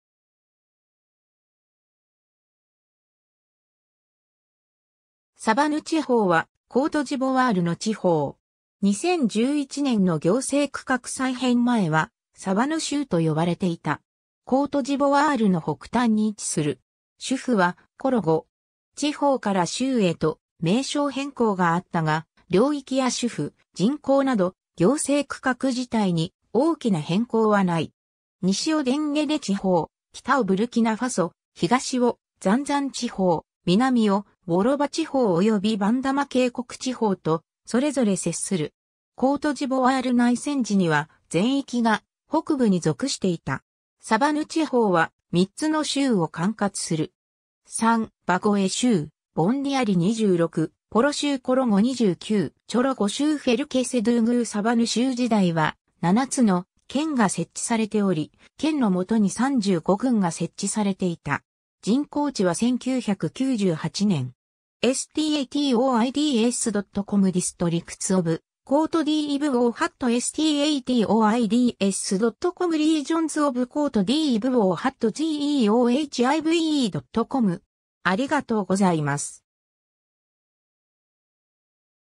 サヴァヌ地方
サヴァヌ地方（Savanes）は、コートジボワールの地方（District）。
2011年の行政区画再編前はサヴァヌ州（Région des Savanes）と呼ばれていた。
コートジボワールの北端に位置する。
首府はコロゴ。
地方から州へと名称変更があったが、領域や首府、人口など行政区画自体に大きな変更はない。
 西をデンゲレ地方、北をブルキナファソ、東をザンザン地方、南をウォロバ地方（旧ウォロドゥーグー州）およびバンダマ渓谷地方と、それぞれ接する。
コートジボワール内戦時には全域が北部に属していた。
 サヴァヌ地方は、3つの州（Région）を管轄する。
 3. バゴウェ州（Bagoué） - ボンディアリ（Boundiali） 26. ポロ州（Poro） - コロゴ（Korhogo） 29. チョロゴ州（fr:Tchologo） - フェルケセドゥーグー（fr:Ferkessédougou） サヴァヌ州時代は、7つの県（Département）が設置されており、県の下に35郡（Sous-Préfecture）が設置されていた。
人口値は1998年。
 ^ Statoids.com Districts of Côte d'Ivoire (Ivory Coast) ^ Statoids.com Regions of Côte d'Ivoire (Ivory Coast) ^ GeoHive.com